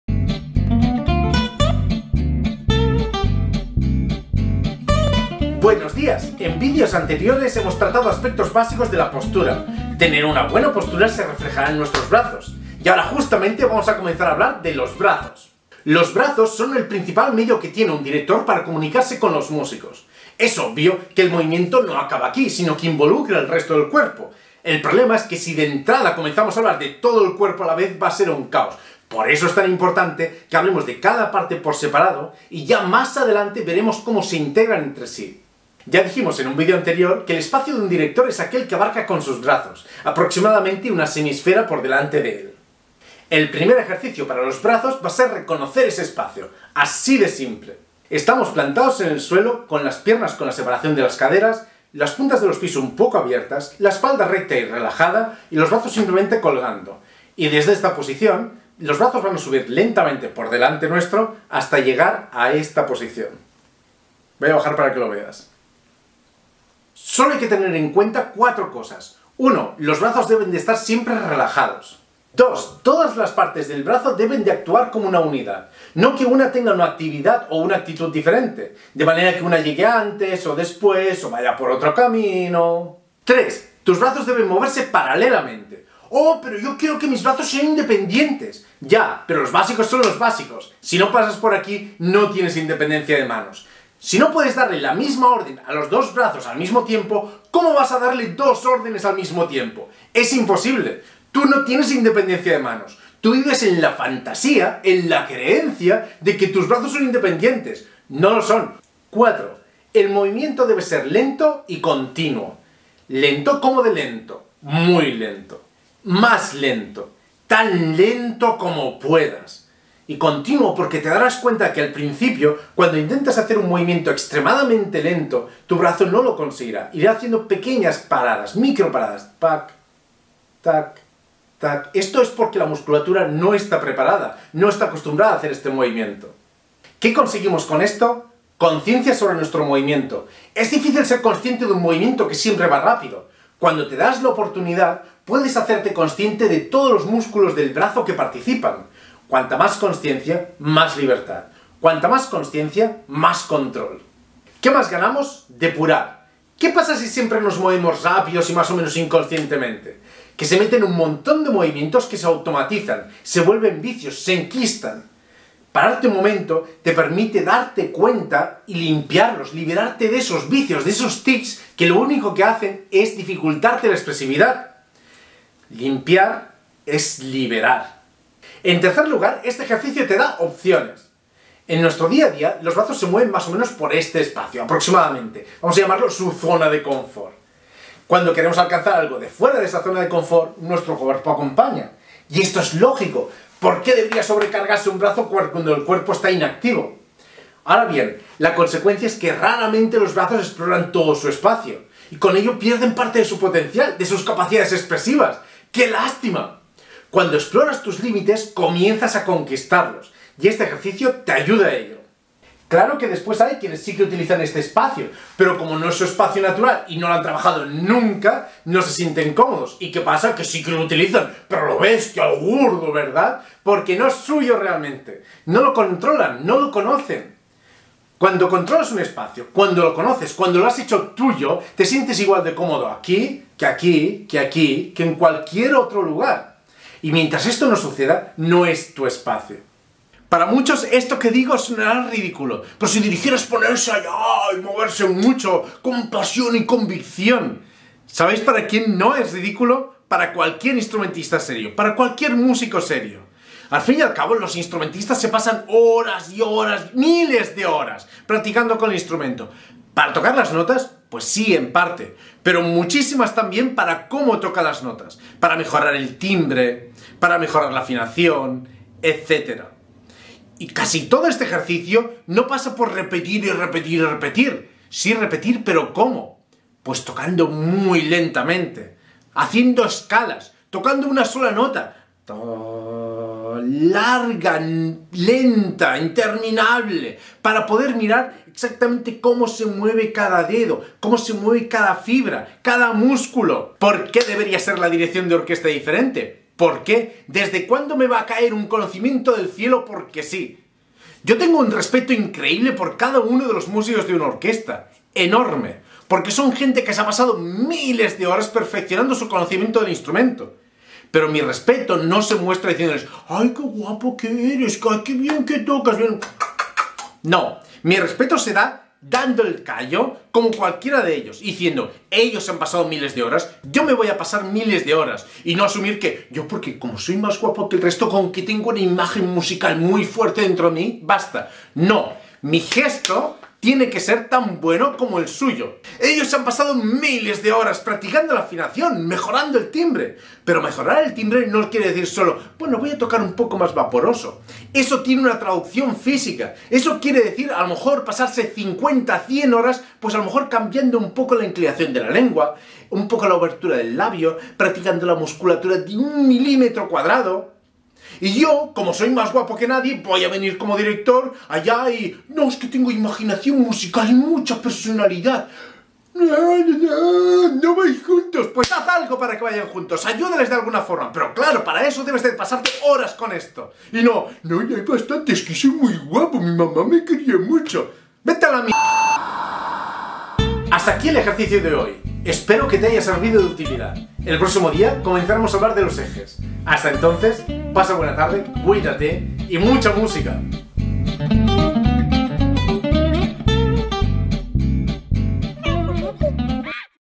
técnica de dirección de orquesta - reconocimiento espacio
ejercicio para familiarizarse con todo el espacio del que dispone el director de orquesta.